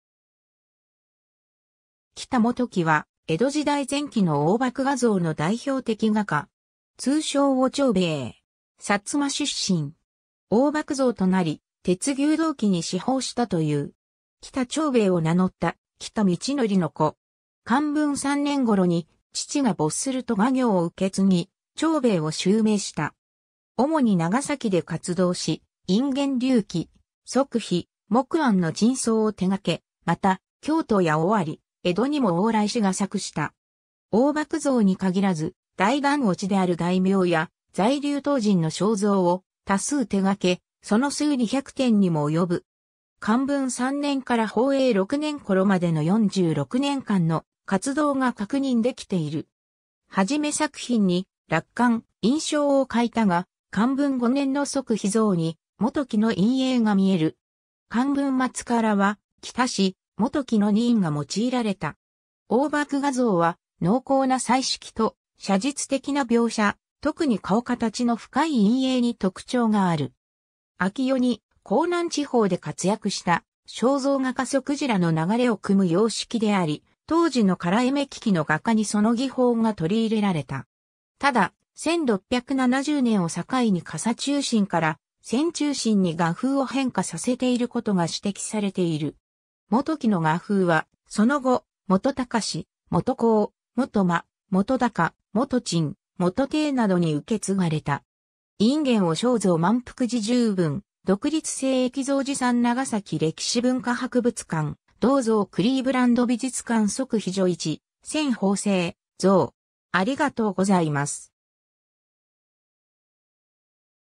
喜多元規
喜多元規（きた げんき、生卒年不詳）は、江戸時代前期の黄檗画像（黄檗僧の頂相）の代表的画家。
 通称を長兵衛。
薩摩出身。
 黄檗僧となり鉄牛道機に嗣法したという。
喜多長兵衛を名乗った喜多道矩の子。
寛文3年頃に父が没すると画業を受け継ぎ、長兵衛を襲名した。
主に長崎で活動し隠元隆琦・即非・木庵の頂相を手がけ、また京都や尾張・江戸にも往来し画作した。
黄檗僧に限らず、大檀越である大名や在留唐人の肖像を多数手がけ、その数200点にも及ぶ。
寛文3年（1663）から宝永6年（1709年）頃までの46年間の活動が確認できている。
 はじめ作品に落款・印章を欠いたが寛文5年の即非像に「元規」の印影がみえる。
寛文末からは「喜多氏」・「元規」の二印が用いられた。
 黄檗画像は、濃厚な彩色と写実的な描写、特に顔貌の深い陰影に特徴がある。
明代に江南地方で活躍した肖像画家曽鯨の流れを汲む様式であり、当時の唐絵目利の画家にその技法が取り入れられた。
ただ、1670年（寛文10年）を境に暈中心から線中心に画風を変化させていることが指摘されている。
元規の画風はその後、元喬・元香・元真・元高・元珍・元貞などに受け継がれた。
 （左）隠元和尚像 萬福寺 重文　　（中左）独立性易像自賛 長崎歴史文化博物館　　（中右）同像 クリーブランド美術館　（右）即非如一・千呆性侒像